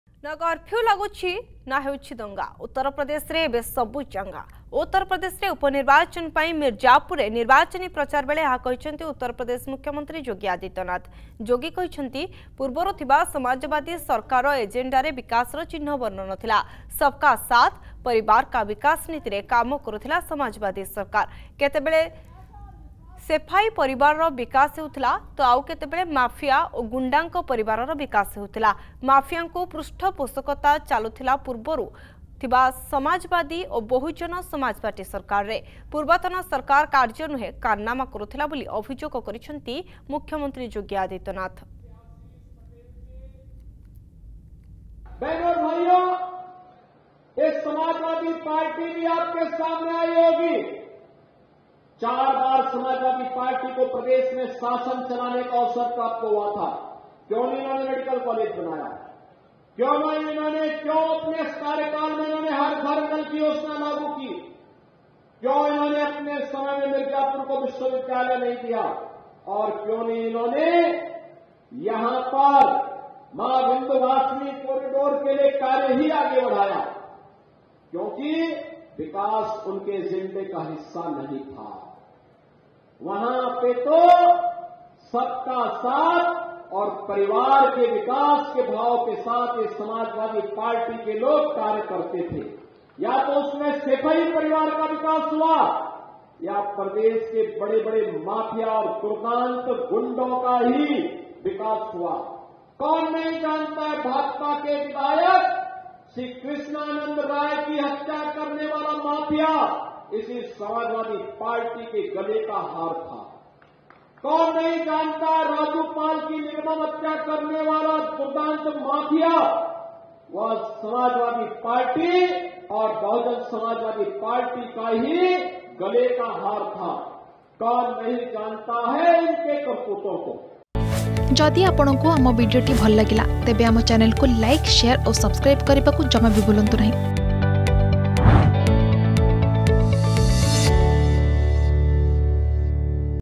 ଉତ୍ତରପ୍ରଦେଶରେ ଏବେ ସବୁ ଚାଙ୍ଗା | CM Yogi adityanath | Argus News
ନା କର୍ଫ୍ୟୁ ଲାଗୁଛି ନା ହେଉଛି ଦଙ୍ଗା । ଉତ୍ତରପ୍ରଦେଶରେ ଏବେ ସବୁ ଚାଙ୍ଗା । ଉତ୍ତ୍ରପ୍ରଦେଶରେ ଉପନିର୍ବାଚନ ପାଇଁ ମିର୍ଜାପୁରରେ ନିର୍ବାଚନୀ ପ୍ରଚାର ବେଳେ ଏହା କହିଛନ୍ତି ଉତ୍ତରପ୍ରଦେଶ ମୁଖ୍ୟମନ୍ତ୍ରୀ ଯୋଗୀ ଆଦିତ୍ୟନାଥ ।